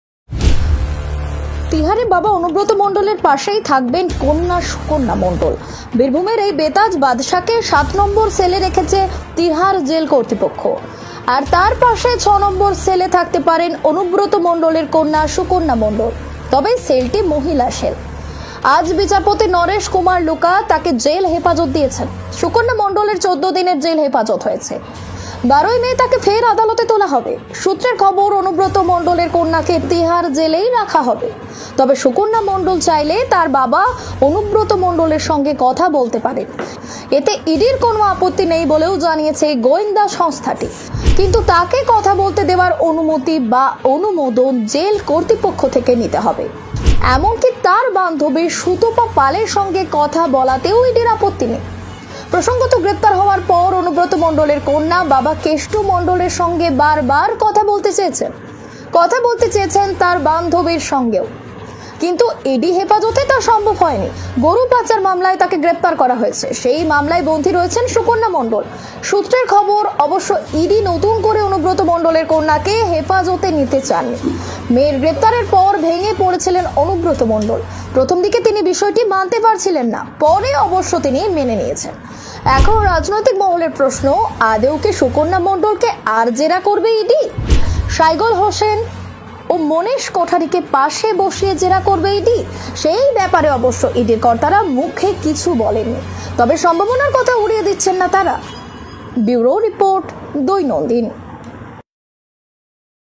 তিহার জেলে বাবা এবং মেয়ে একইসঙ্গে থাকবেন অনুব্রত মণ্ডল এবং সুকর্ণা মন্ডল
tmc Anubrata Mondal# sukarna Mandal#

nhmedia786
 বাবা এবং মেয়ে তিহার জেলে